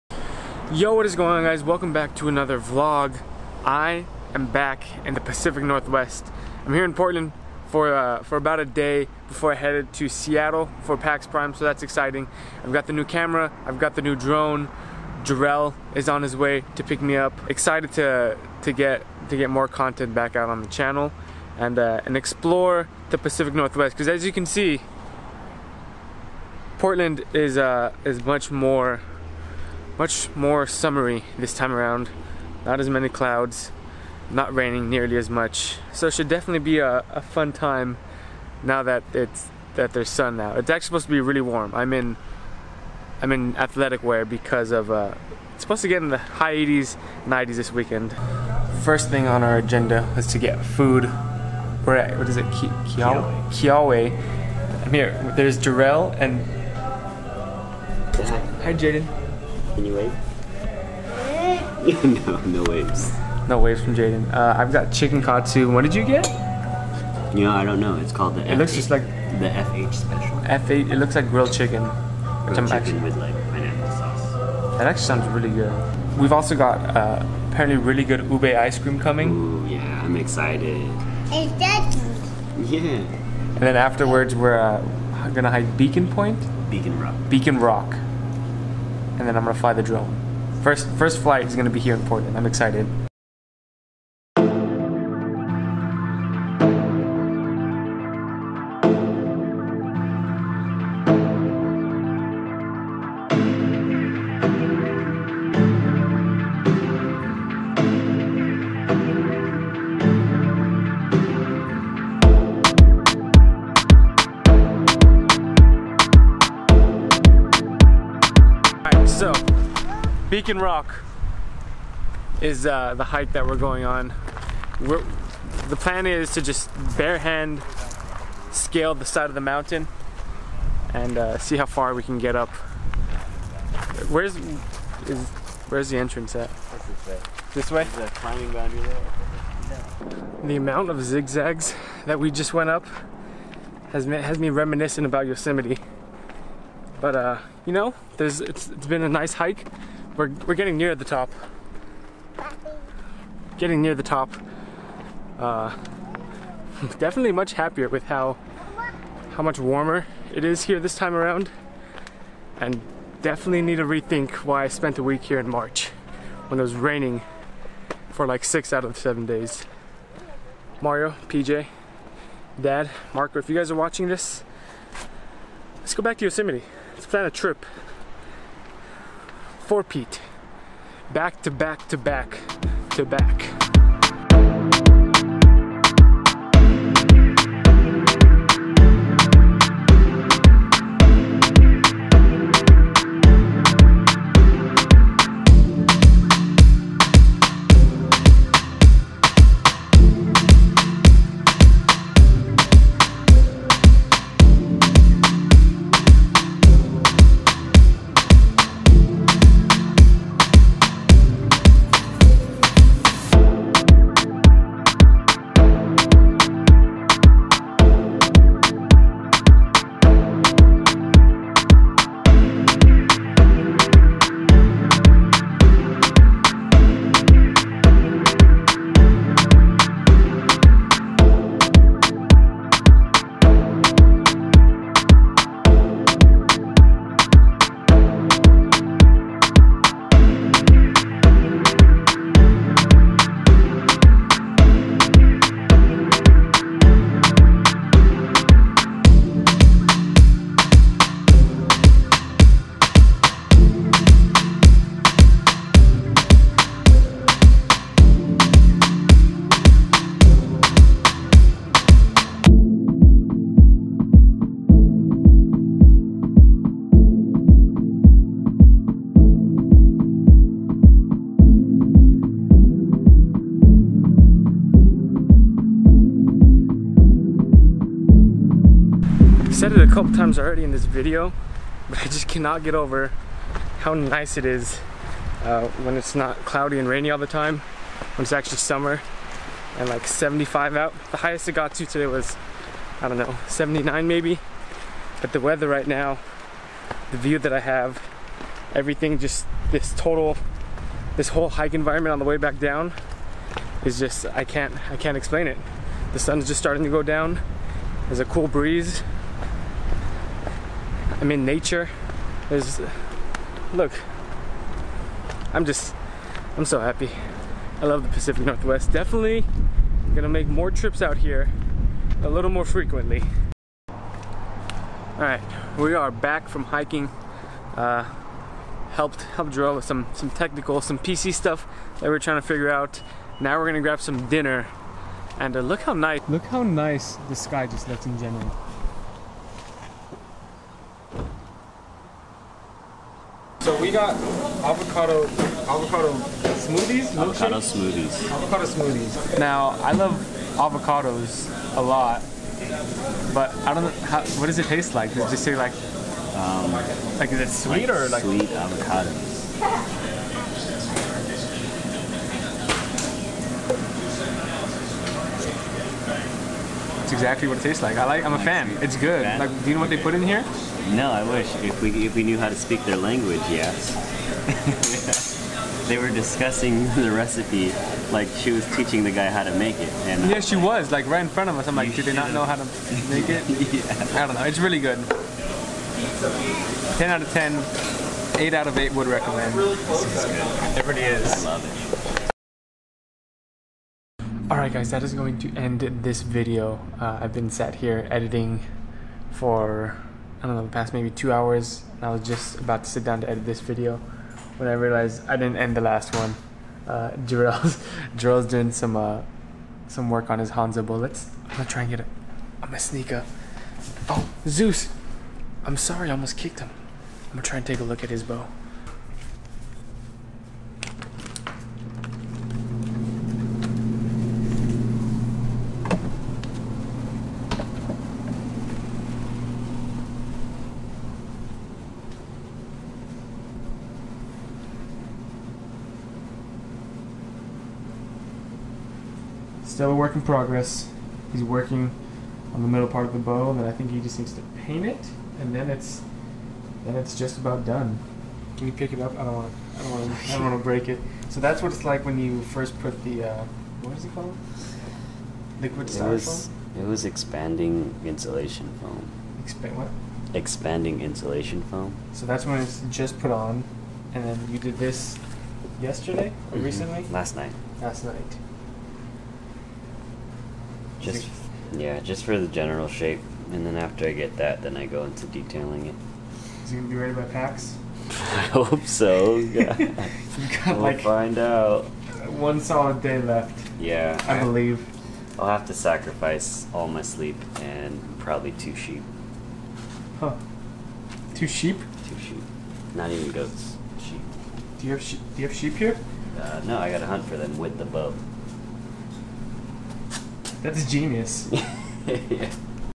Beacon Rock Hike ft. Jerell | Vancouver Vlog
Spending a few days in Vancouver before PAX West & spent some the day w/ Jerell. Food, Hiking, & more food lol
So so happy to be back in the PNW :D